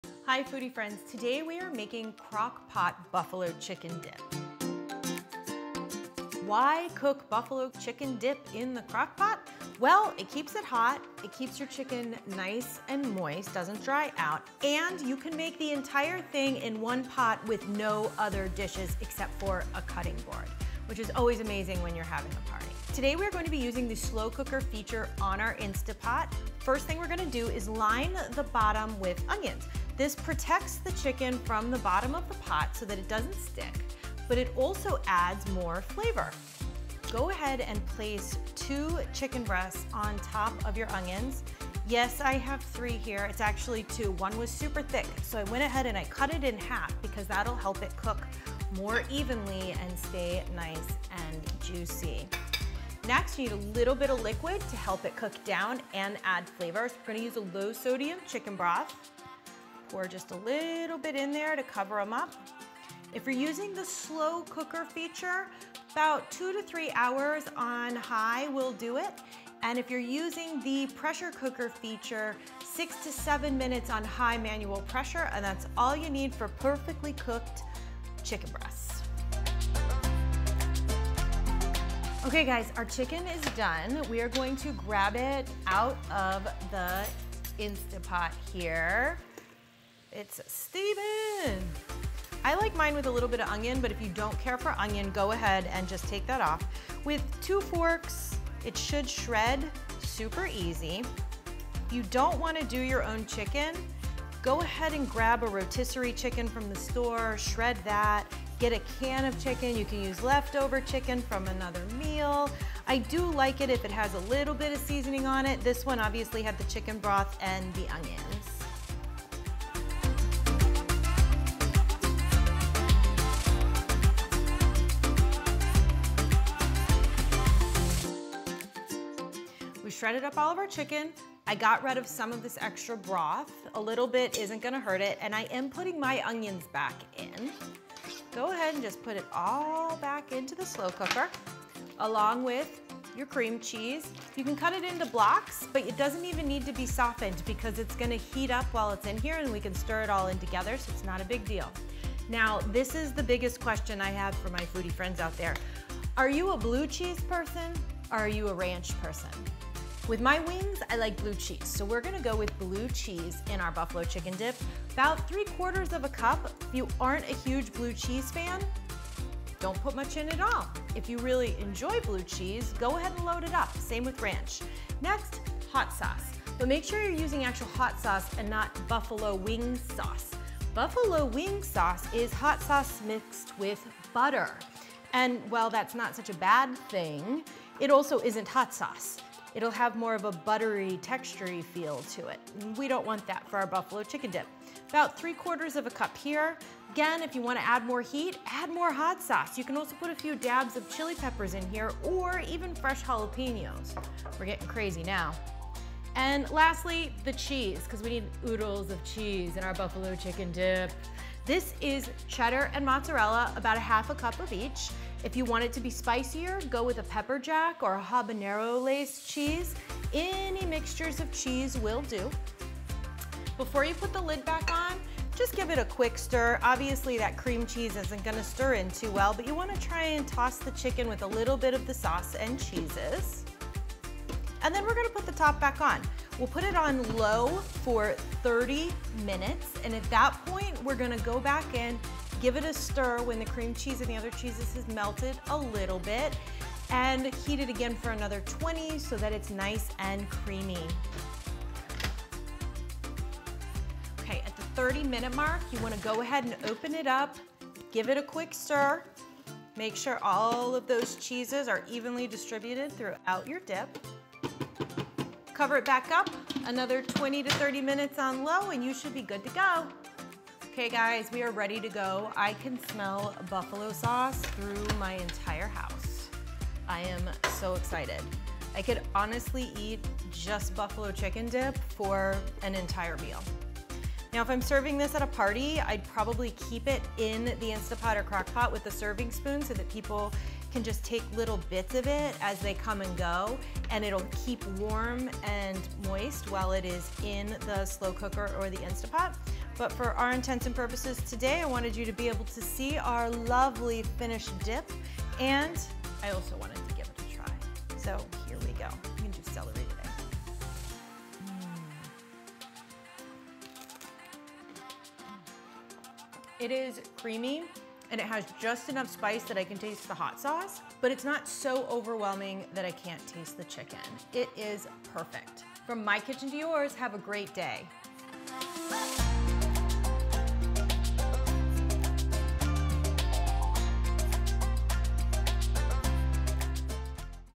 BEST Slow Cooker Buffalo Chicken Dip!
What goes hand in hand with football season? Football snacks! And this Slow Cooker Buffalo Chicken Dip is my all time favorite! Watch as I teach you how to make it step by step, and you will be surprised at how easy it truly is! I answer questions like:
How to cook the chicken?
Can you use an Instant Pot instead?
How to shred chicken?

Some of our favorite tools (affl):
Slow Cooker
Instant Pot